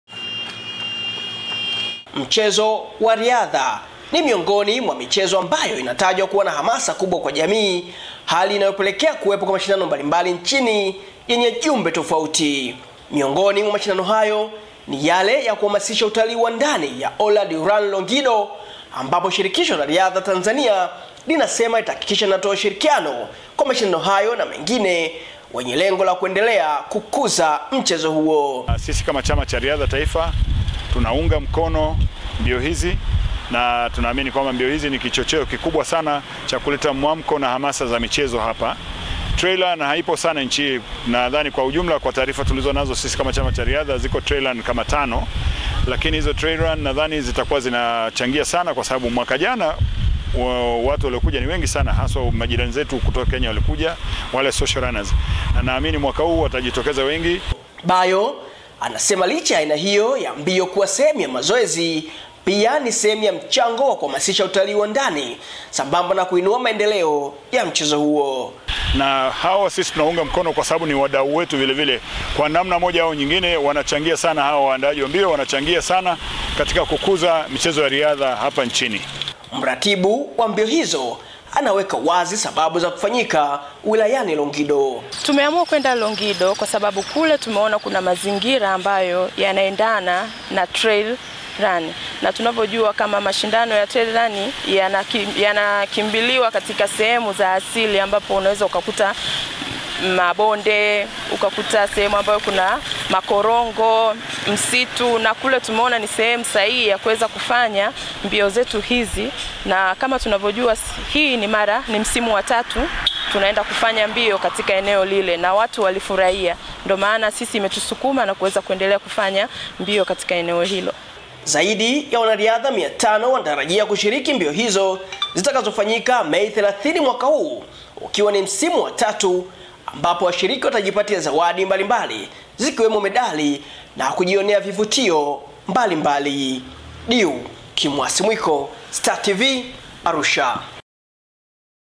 RT WAJA NA MPYA KWENYE UTALII.
Makamu wa Rais wa shirikisho la Riadha Tanzania RT John Bayo amesema shirikiksho hilo litahakikisha linakuwa sehemu ya kukuza mchezo wa riadha nchini kwa kushirikiana na wadau mbalimbali  wanaoandaa mbio zitakazogeukia  kutangaza utalii wa ndani.